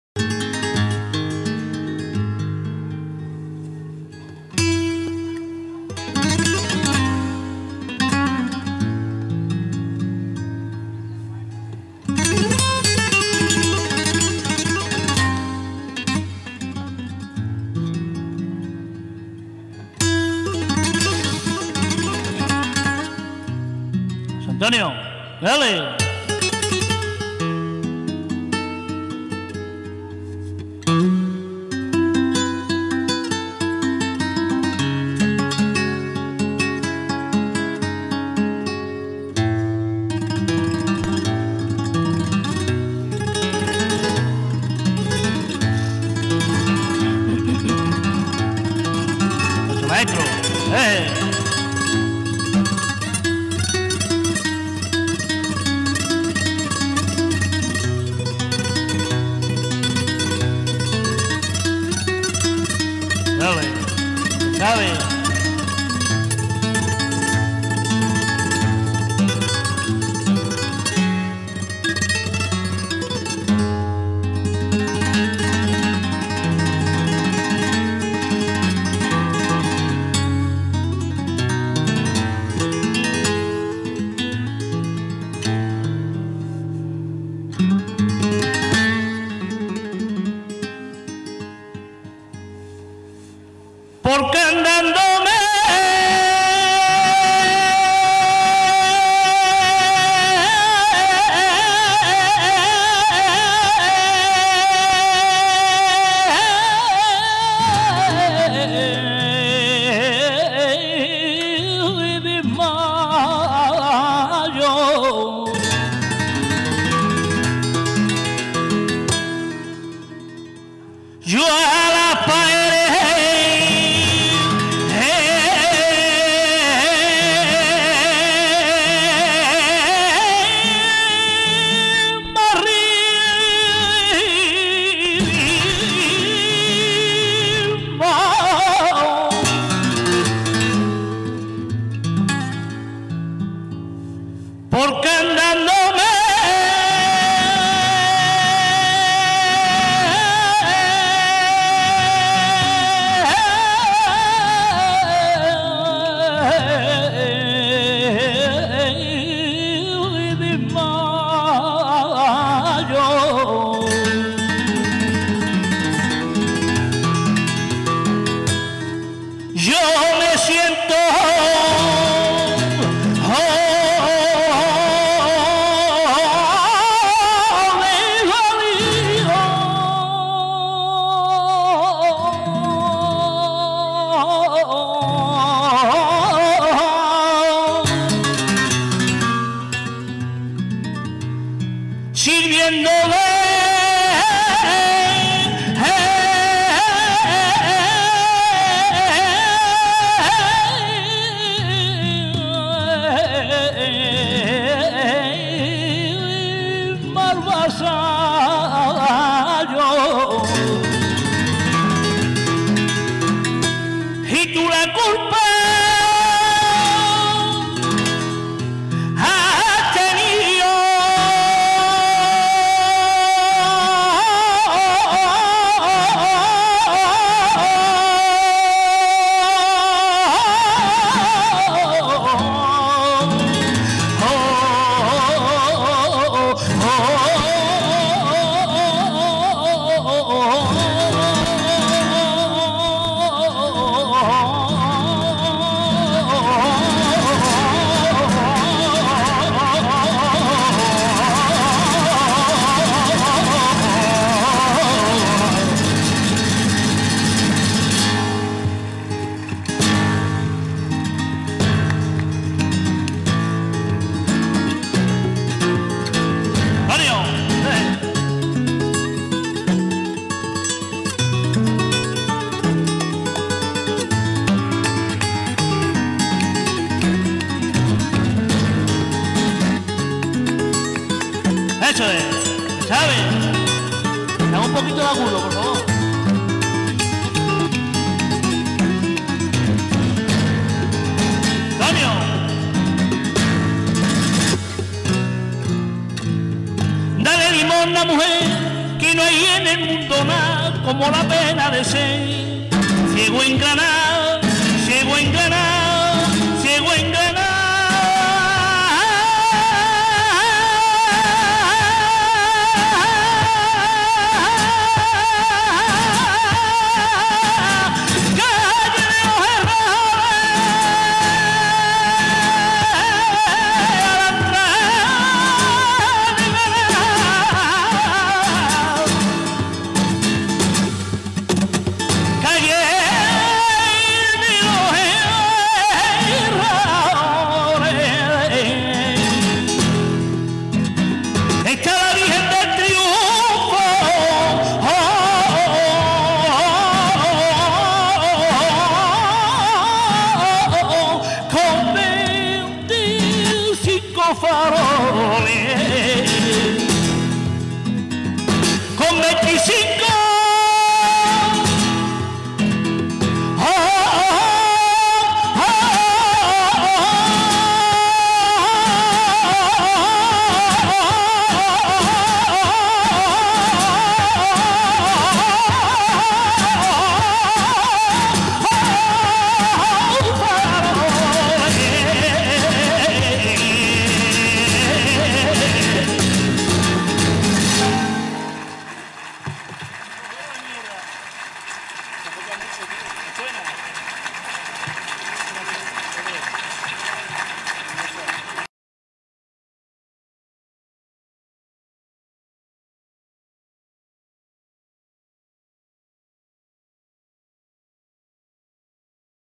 Rubito de Para Hijo y Antonio Caceres por malagueñas abandolao - Carmona XXIV Noche Flamenca
Actuacion del cantaor Rubito de Para Hijo, acompañado a la guitarra por  Antonio Caceres cantando por malagueñas abandolao.

Carmona XXIV Noche Flamenca.